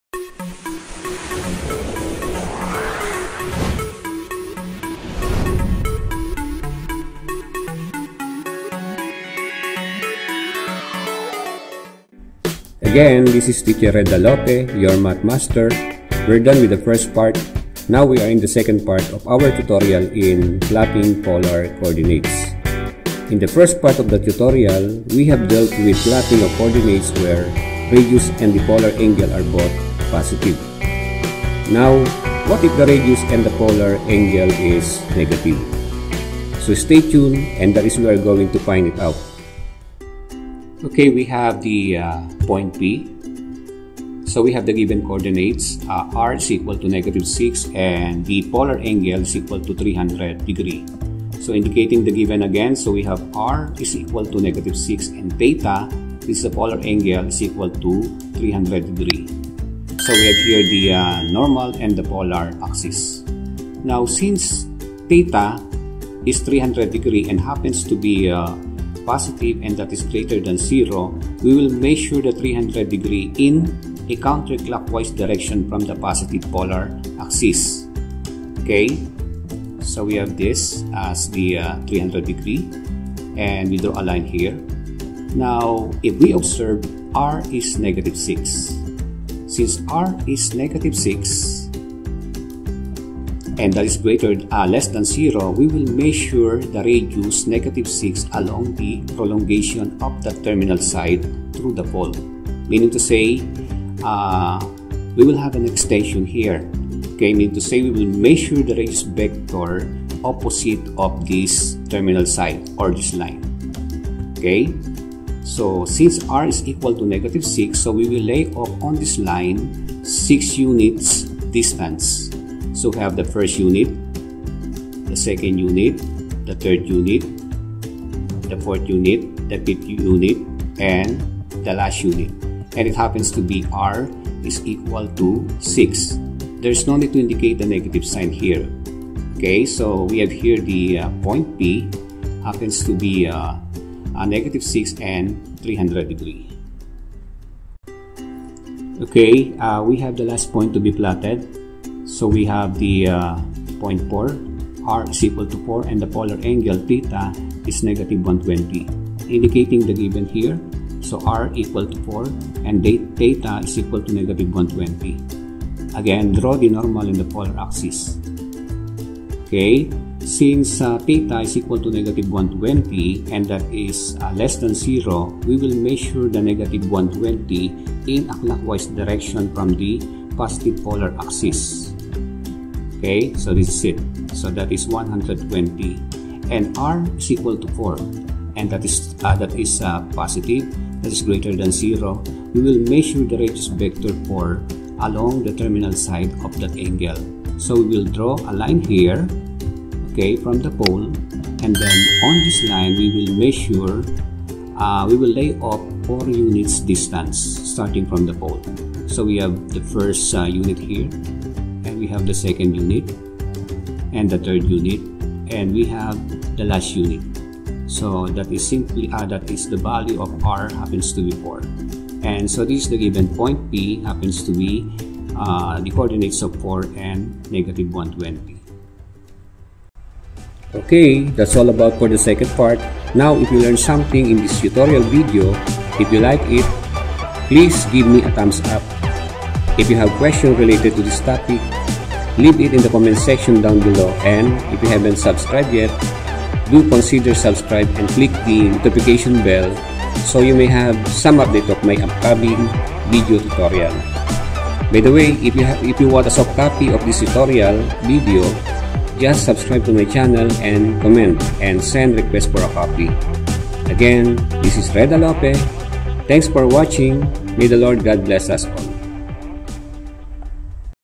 PLOTTING OF POINTS USING POLAR COORDINATES (Part 2)
This video tutorial (Part2) provides basic steps on how to graph Polar Coordinates by plotting points using the radius of the circle and Polar angle measured in the positive polar axis
If you would like to learn on how to plot points, using Polar Coordinates then this video is for you! I am going to show you how to quickly and easily plot points. Make sure to watch the video so it will guide you on how to do it properly